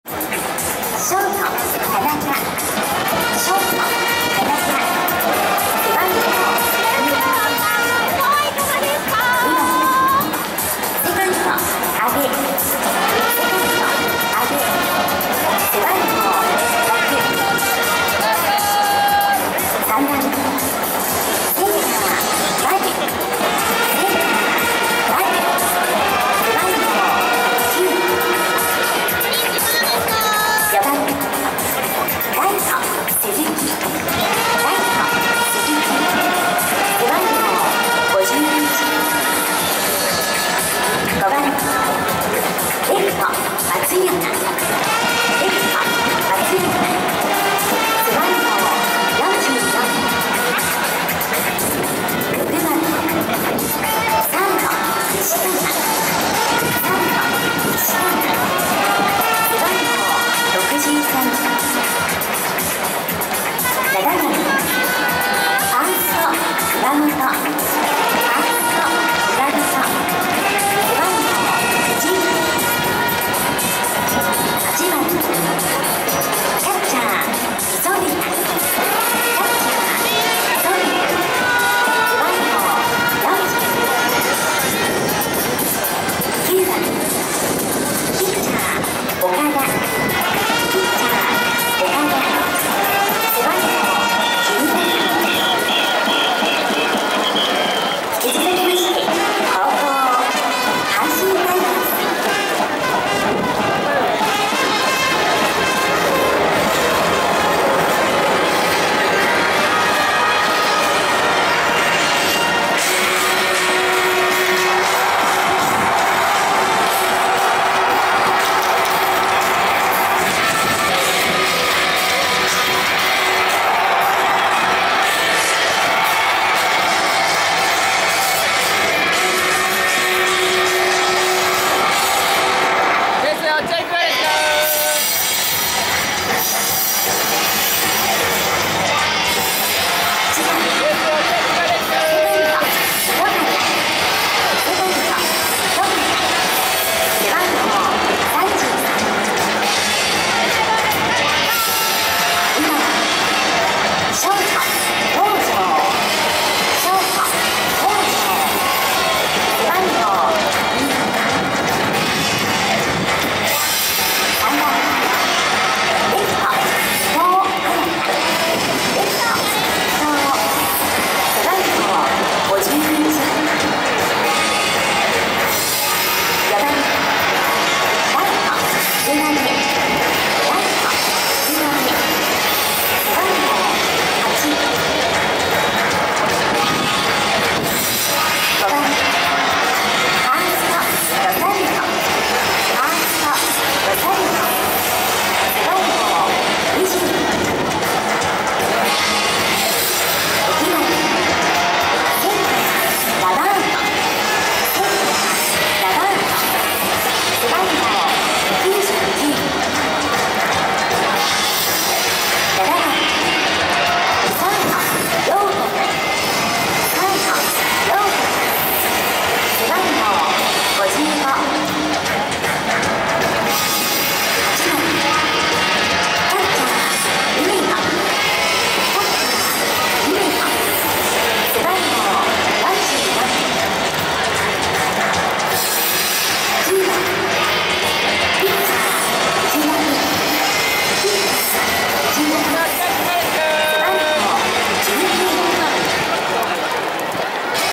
阪神VS広島戦 H30 7 26 甲子園球場 ウル虎の夏 両チームスタメン
H30 7 26、阪神タイガース、死のロード前の阪神甲子園球場最後の試合、ウル虎の夏、阪神タイガースVS広島カープのスタメン。この日は結局、試合直前の大雨で試合時間が1時間10分程度遅れ、試合が最終的に終わったのが10時40分頃。おまけに1回表に藤浪晋太郎が、2アウトしか取れず早々にノックアウト。広島カープに3対9で惨敗しました。